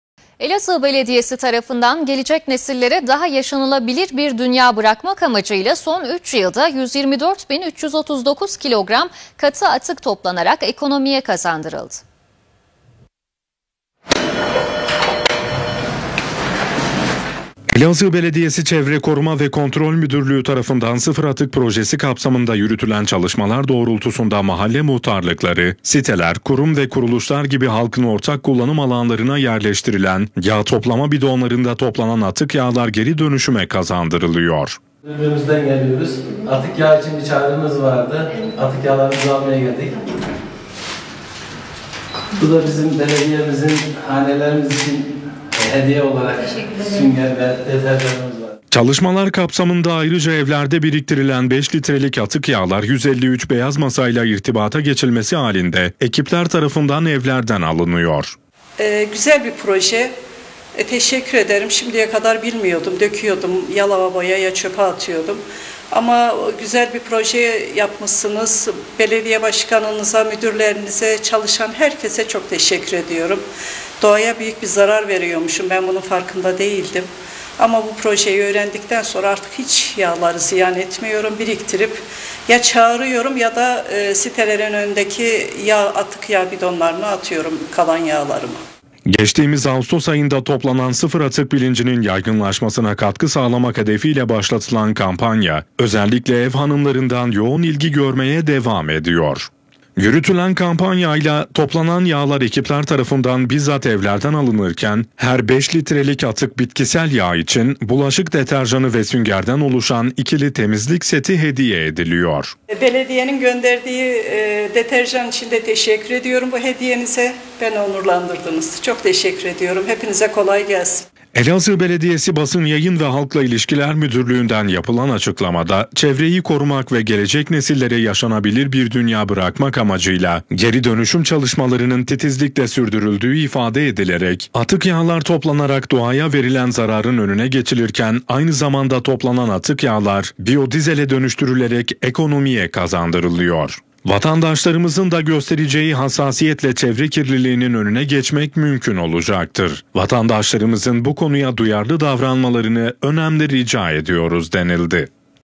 Basında Biz | Kanal 23 “Elazığ Belediyesi’nden Çevre Duyarlılığı”
Elazığ Belediyesi, gelecek nesillere daha yaşanabilir bir dünya bırakmak hedefiyle çevresel yatırımlarını sürdürüyor. Atıkların geri dönüşüme tabi tutularak ekonomiye kazandırılması kapsamındaki çalışmalar doğrultusunda, son üç yılda 124.339 kg atık bitkisel yağ toplanarak ekonomiye kazandırıldı.